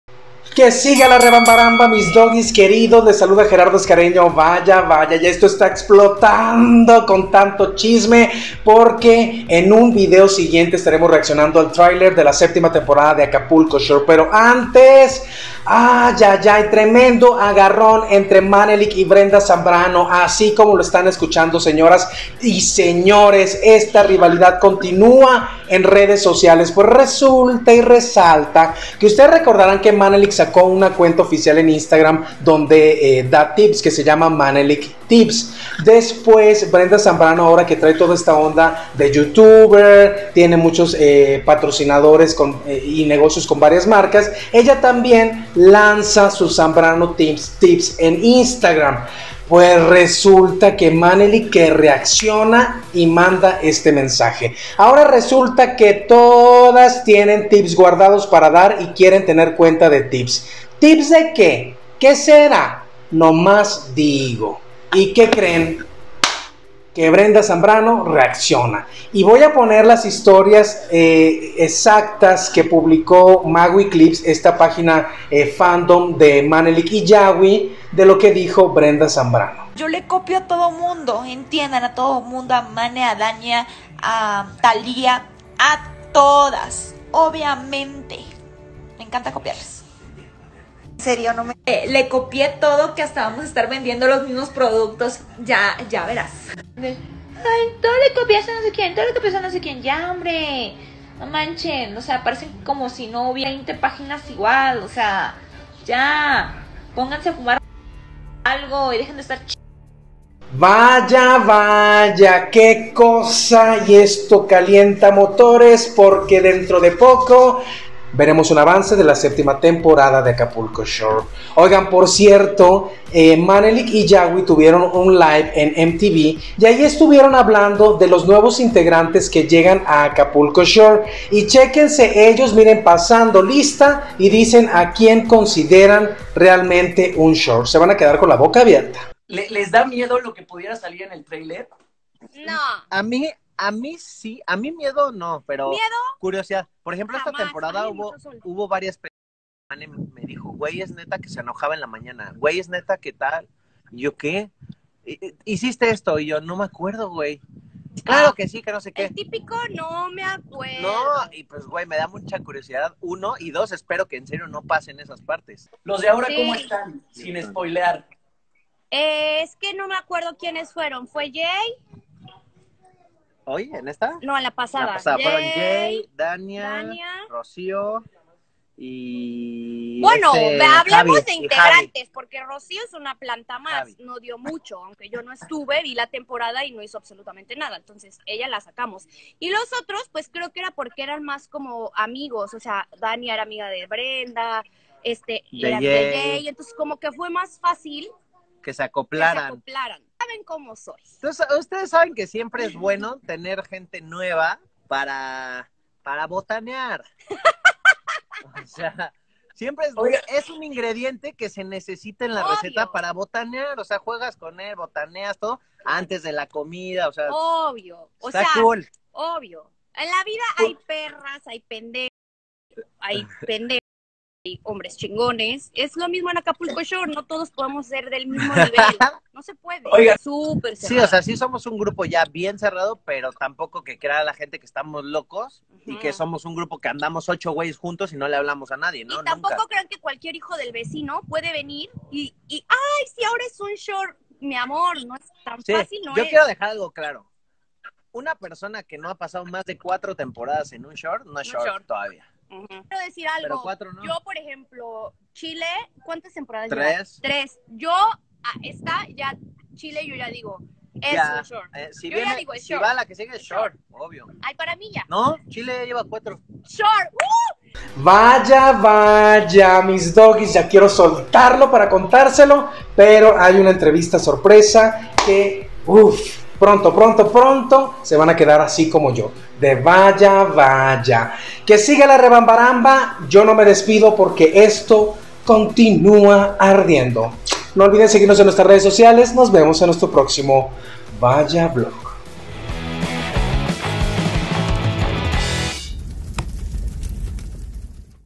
Vaya Vaya 🤔 : Manelyk y Brenda Zambrano broncón por tips
Ya hay fecha oficial para Acapulco Shore 7, será el martes 7 de junio.

**Vaya Vaya**